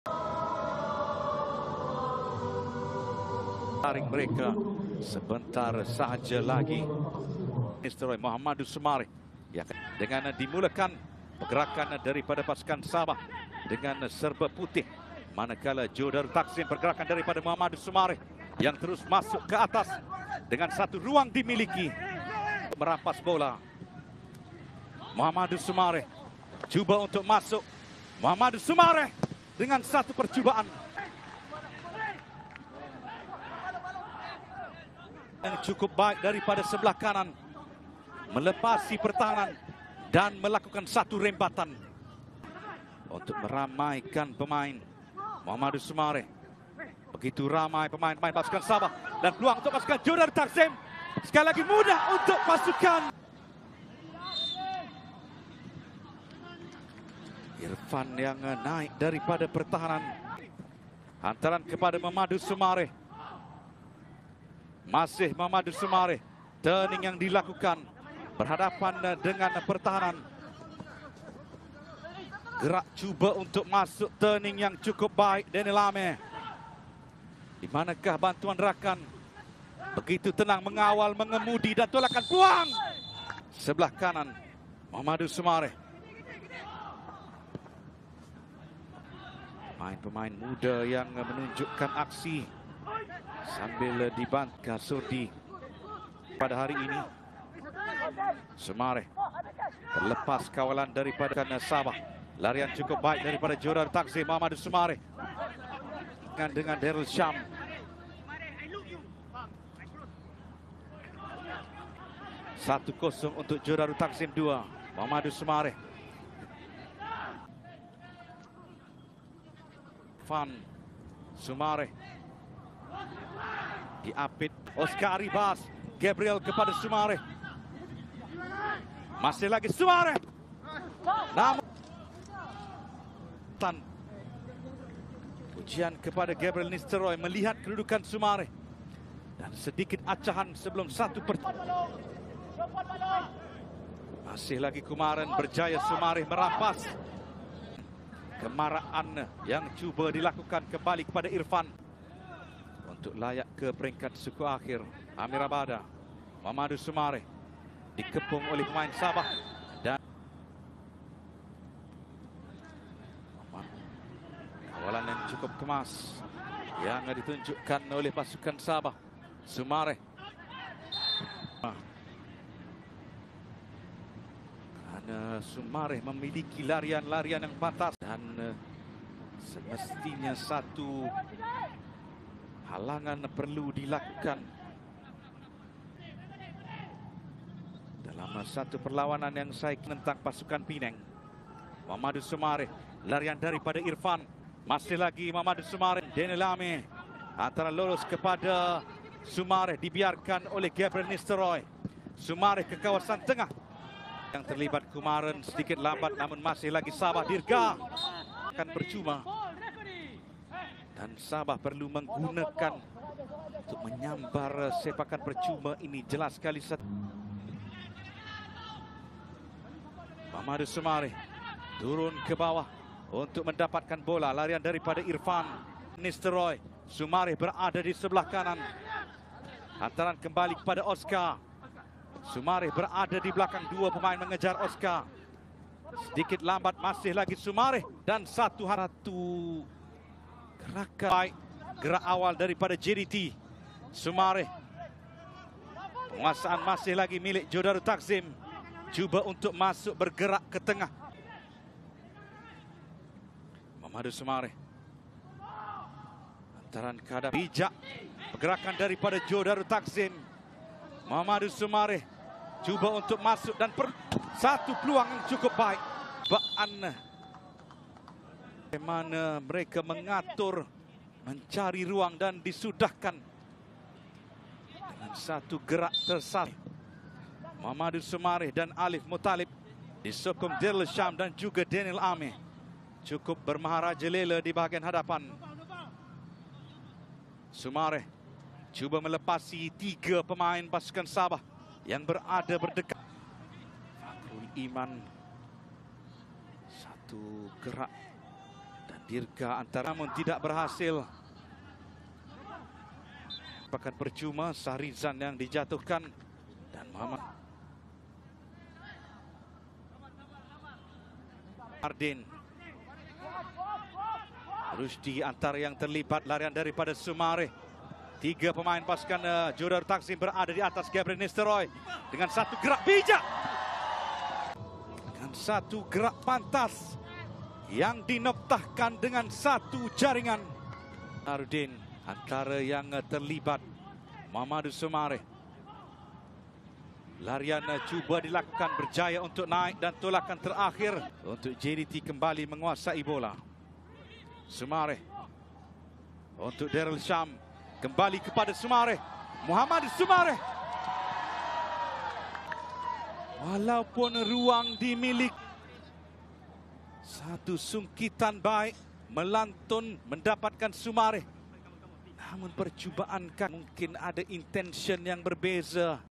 Mohamadou Sumareh (31) Vs Sabah FC• Keep Going Just Don't Forget How Good Sumareh Is
Liga Premier Malaysia 2023

Personal Highlight For Mohamadou Sumareh 13, Winger Johor Darul Ta'zim and Malaysia National Football Team.